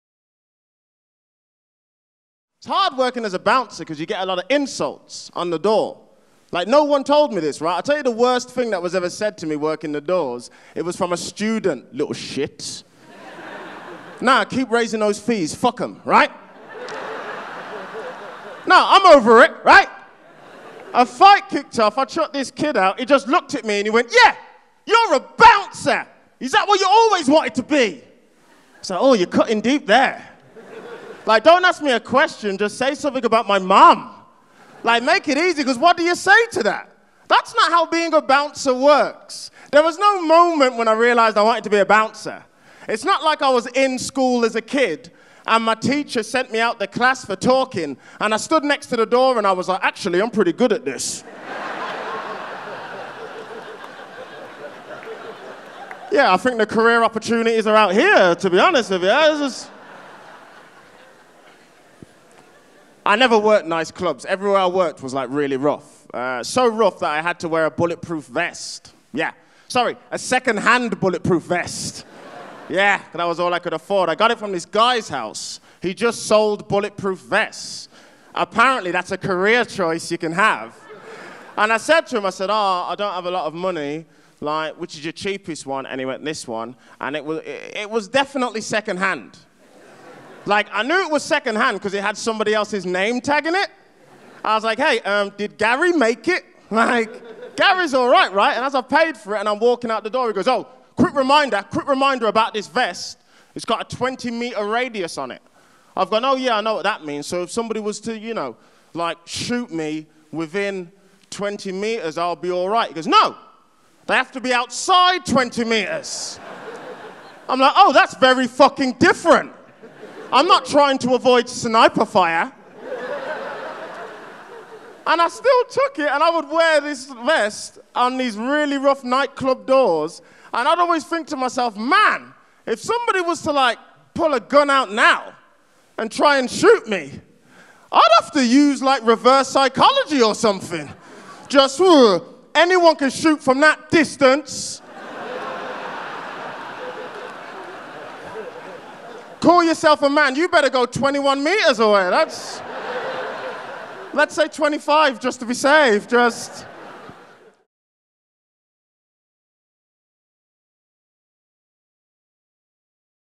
Leicester Comedy Festival - Gala Preview Show - Darren Harriott
Darren Harriott takes to the stage at De Montfort Hall for Leicester Comedy Festival's annual Gala Preview Show, 2019.